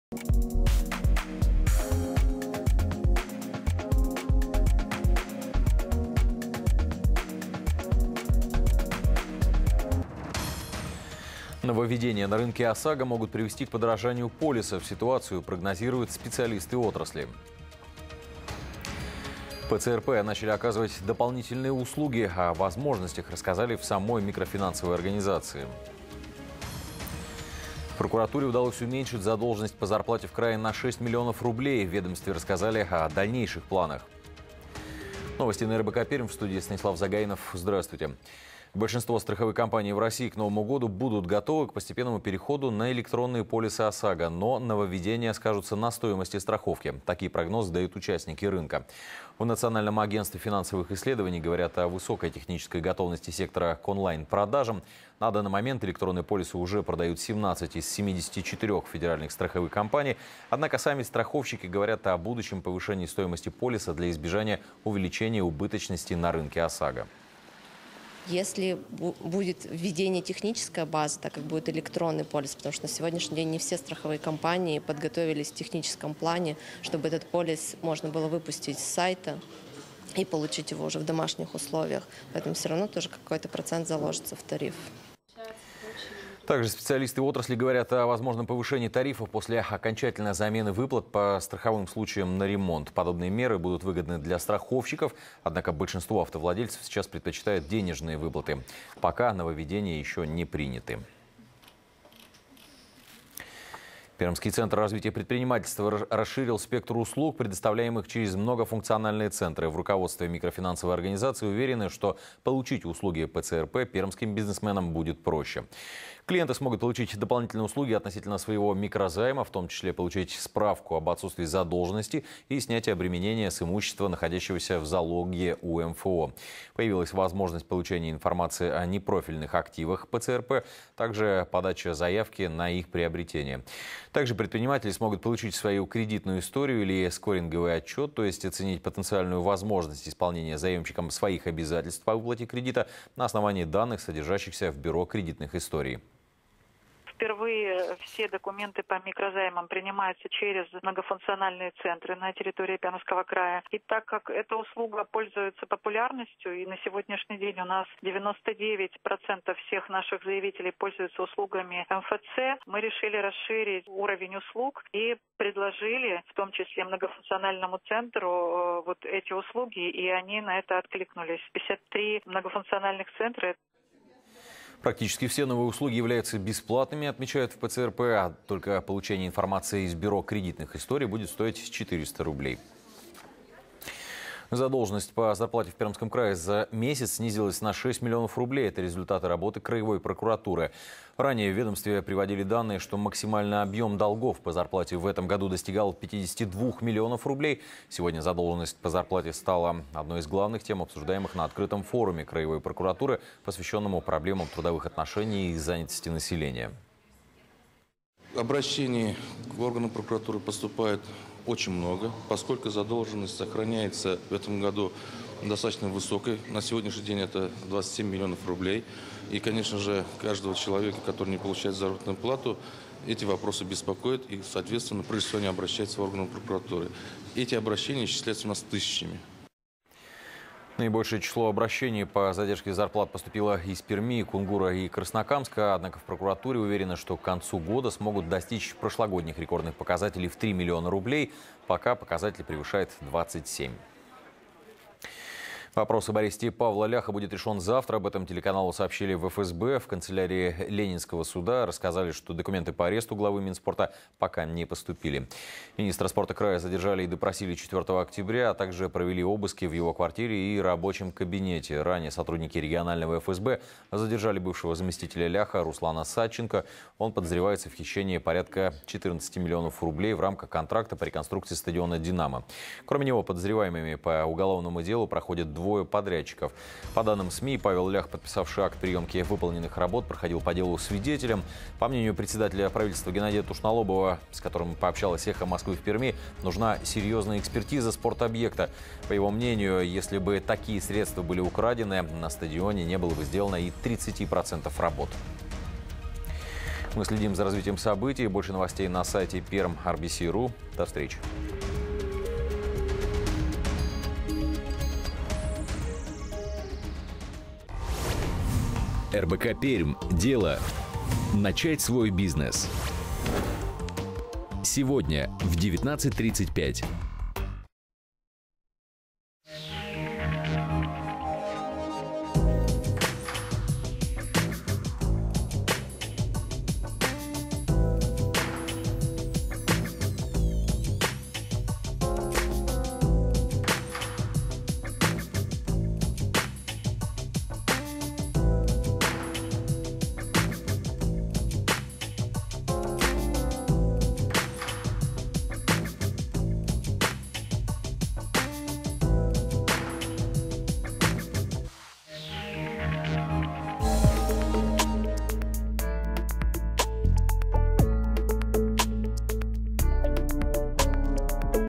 НОВОСТИ ПРИКАМЬЯ 05.10.16 18-30
Темы выпуска:
- Эксперты: в этом году начать продавать электронные полисы ОСАГО успеют практически все игроки рынка.
- ПЦРП: практически все новые услуги, оказываемые в МФЦ, будут бесплатными.
- Прокуратура края намерена до конца года снизить задолженность по зарплате до 3 млн. рублей.

Ведущий выпуска: Станислав Загайнов